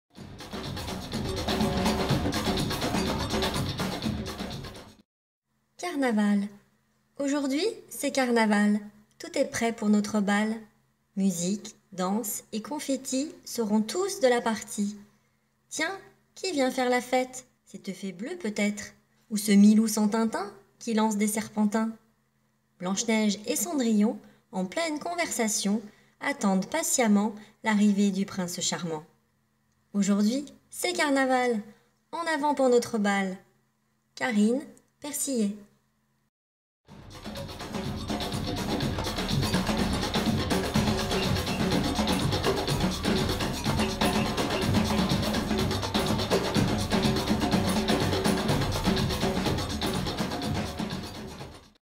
Poésie 🎊 Carnaval de Karine Persillet 🎊
Voici une petite vidéo qui t'aidera à apprendre la poésie (poème) : en l'écoutant et en la lisant en même temps.

Bon visionnage !

Carnaval
Aujourd’hui, c’est Carnaval !
Tout est prêt pour notre bal.
Musique, danse et confettis
Seront tous de la partie.

Tiens, qui vient faire la fête ?
Cette fée bleue peut-être,
Ou ce Milou sans Tintin
Qui lance des serpentins !

Blanche-Neige et Cendrillon
En pleine conversation
Attendent patiemment
L’arrivée du prince charmant.

Aujourd’hui, c’est Carnaval !
En avant pour notre bal !
Karine Persillet

Merci !💚

Pas d'attribution requise
Autoriser pour usage commercial.